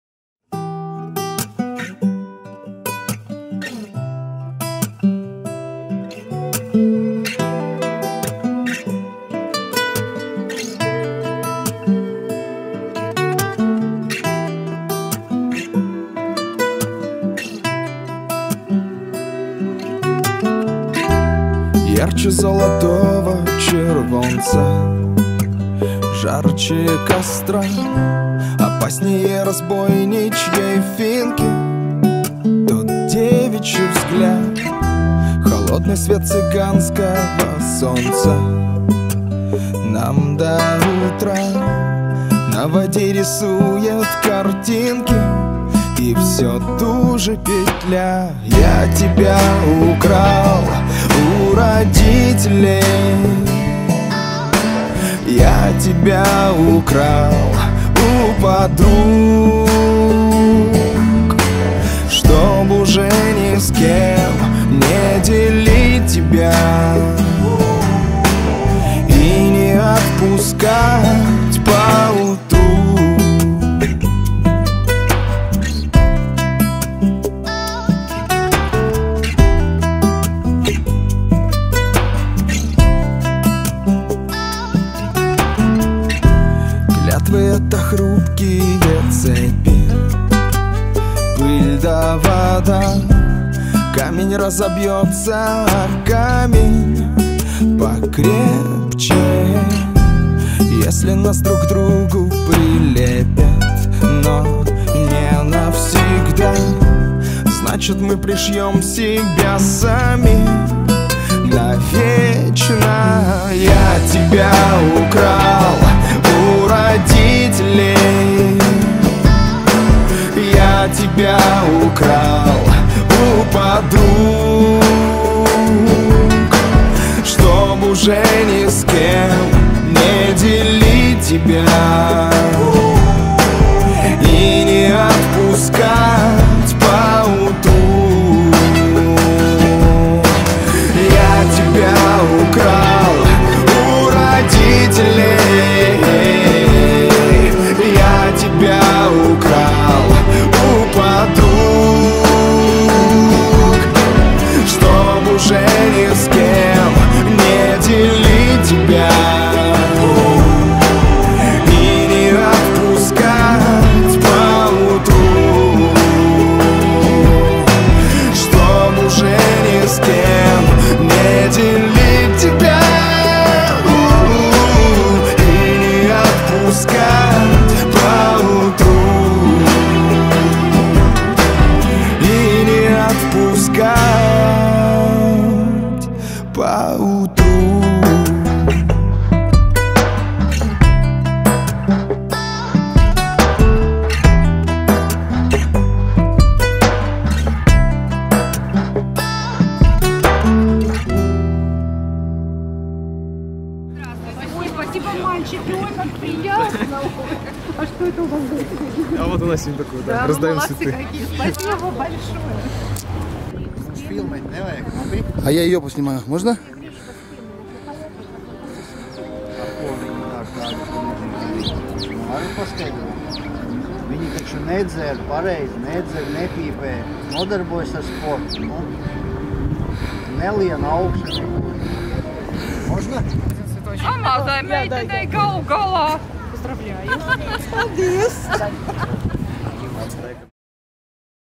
С 8 марта!
Дорогие дамы! Tanec FILM поздравляет Вас с 8 марта и желает оставаться прекрасными.